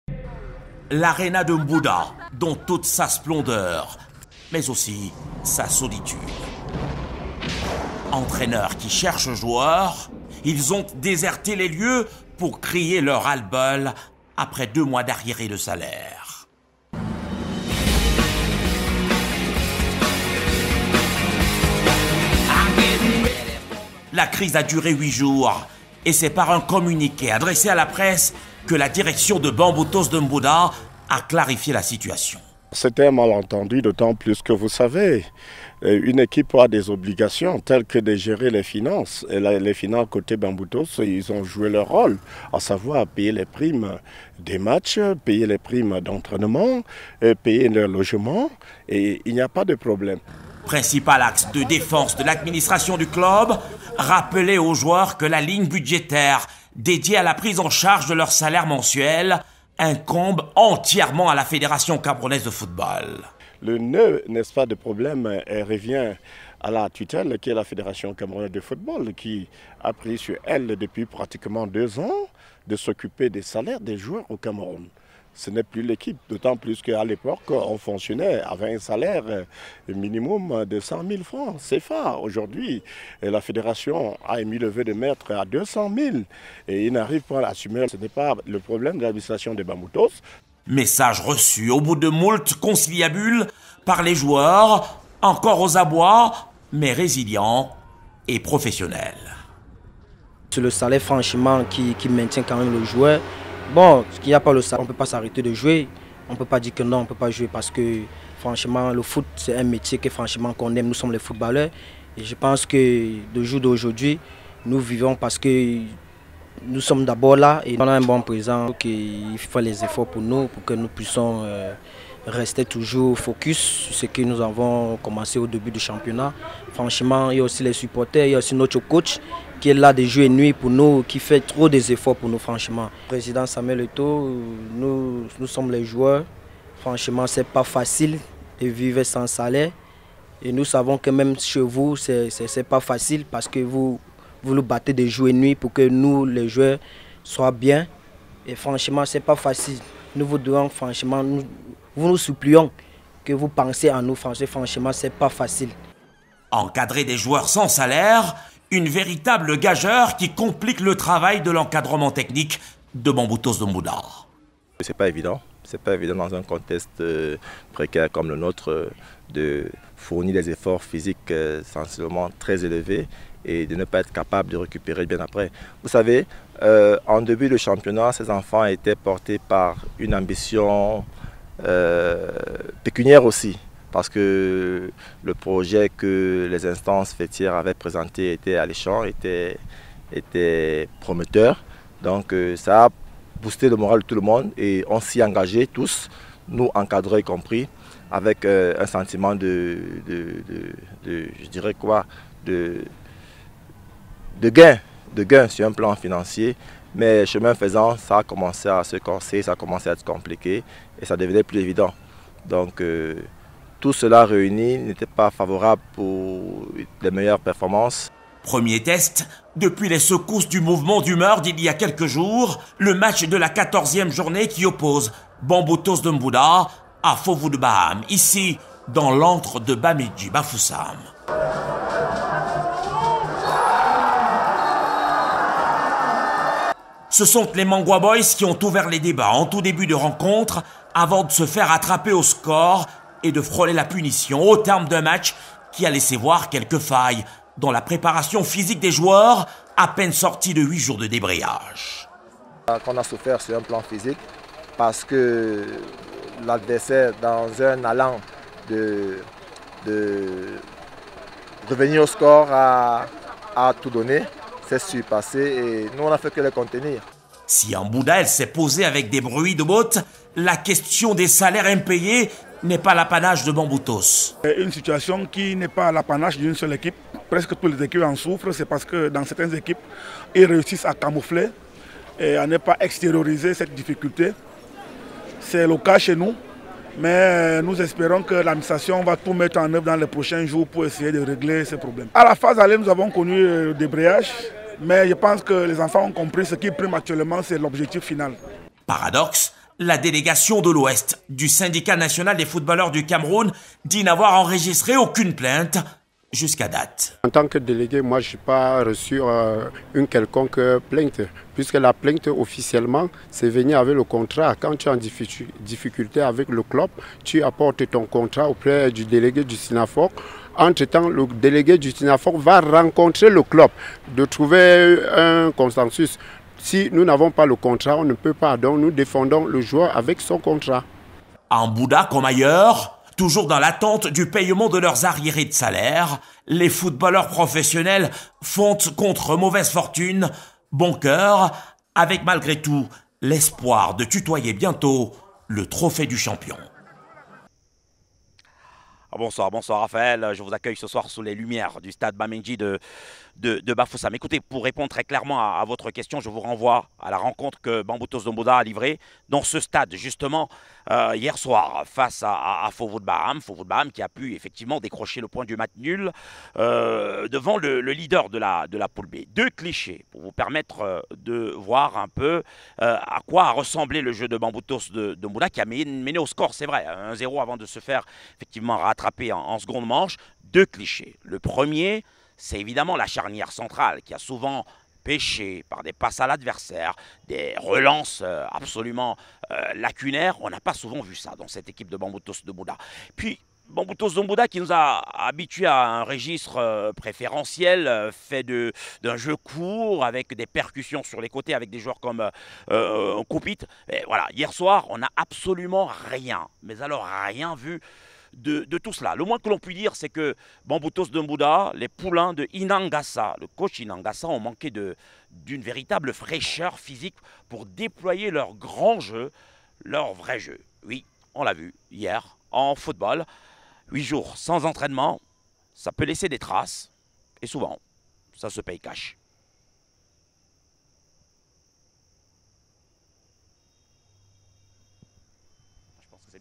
MOUVEMENT D'HUMEUR : LES JOUEURS DE BAMBOUTOS BOUDENT LES SÉANCES D'ENTRAÎNEMENT
Pour la rubrique "Grand reportage" de Fou Fou Fou, Luc Perry Wandji vous immerge dans les méandres de Bamboutos de Mbouda.